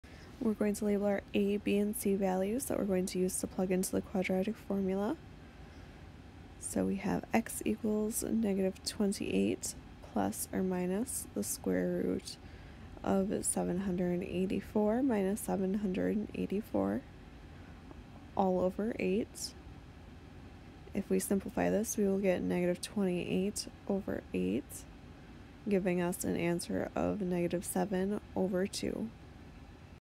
Quadratic Equation: Solve 4x^2 + 28x + 49 = 0
Tiala from SVSU Micro Math helps you solve a quadratic equation using the quadratic formula.  This equation has one repeated rational solution.

Problem: Solve 4x^2 + 28x + 49 = 0